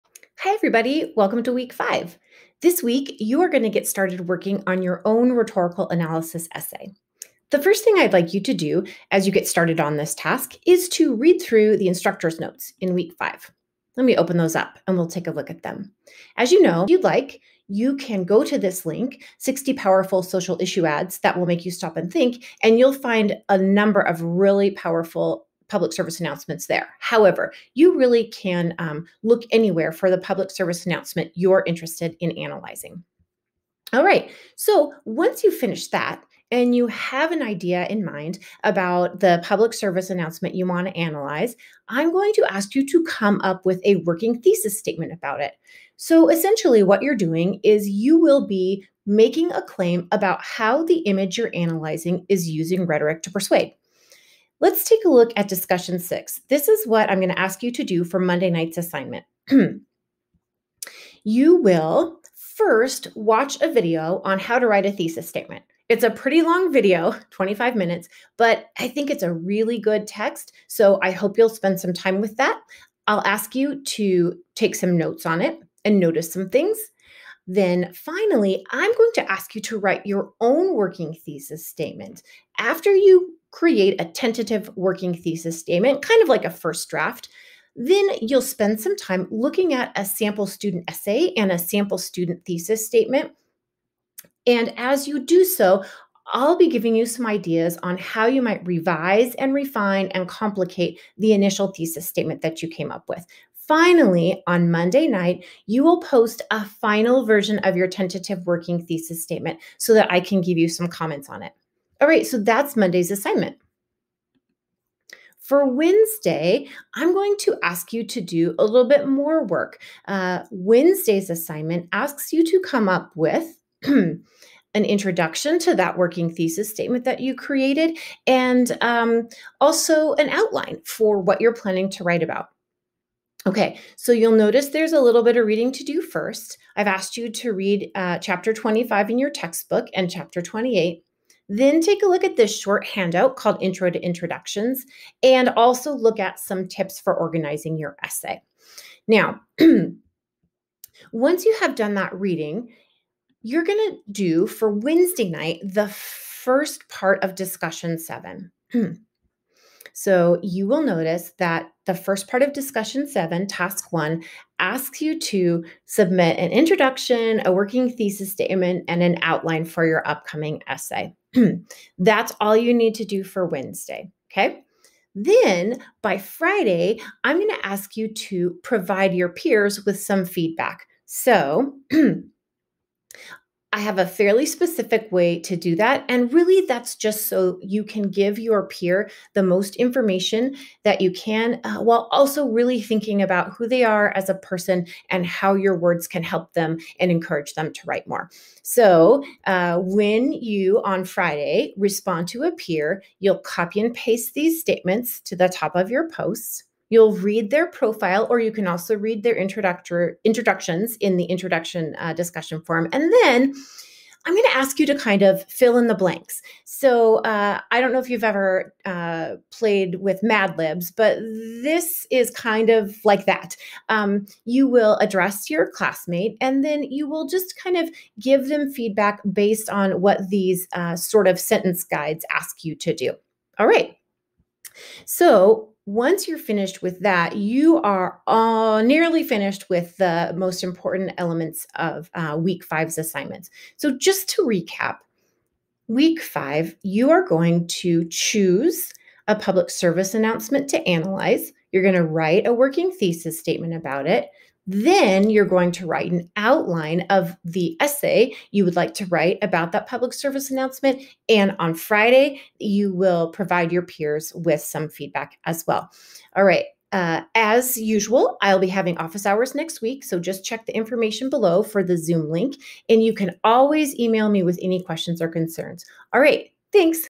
Welcome to Week Five
Jessica describes Week 5 assignments: choose a psa, write a working thesis and outline, provide peer response